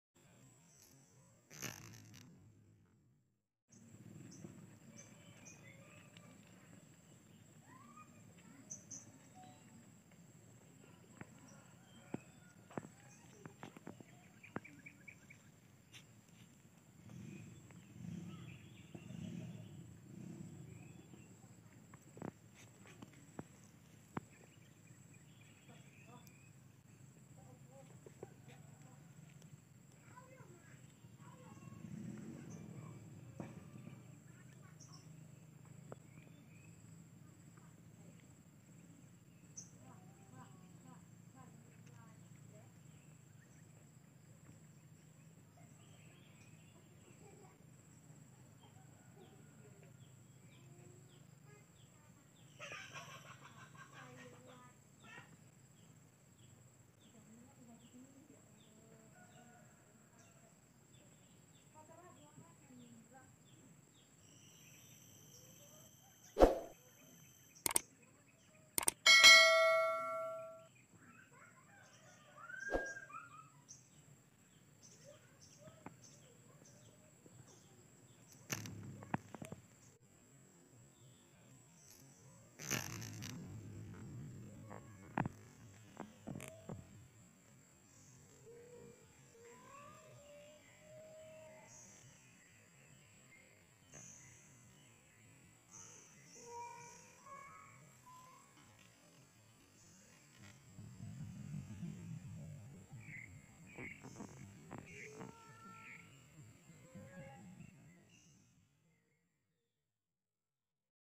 Slow Motion Rhinoceros Beetle Take Flight
The Big Flying Beetle shows the best moments, especially seeing with slow motion, this type of beetle is a female rhinoceros beetle. His favorite food is cucumber, every day he really likes to eat cucumber.

Do you like beetles? On this All Animals Channel we present many videos about beetles.